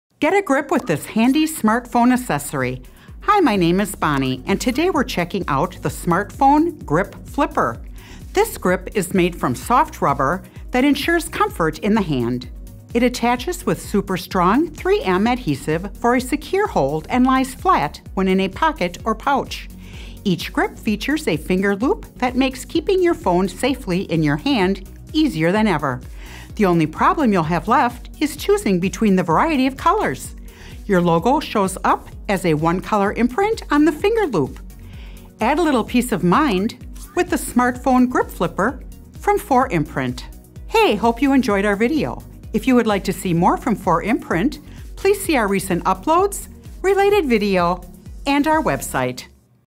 Smartphone Grip Flipper - Custom Phone Grip by 4imprint Canada
This imprinted smartphone grip is made from rubber for a soft feel. Each grip features a finger loop for easier smartphone handling. For easier storage, the finger loop lies flat when in a pocket or pouch. Grip flippers are complete with a strong, removable 3M® adhesive. Smartphone grip flippers are the perfect accessory for their device! Your price includes a one colour imprint on the finger loop.

to learn more or call toll free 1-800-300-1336.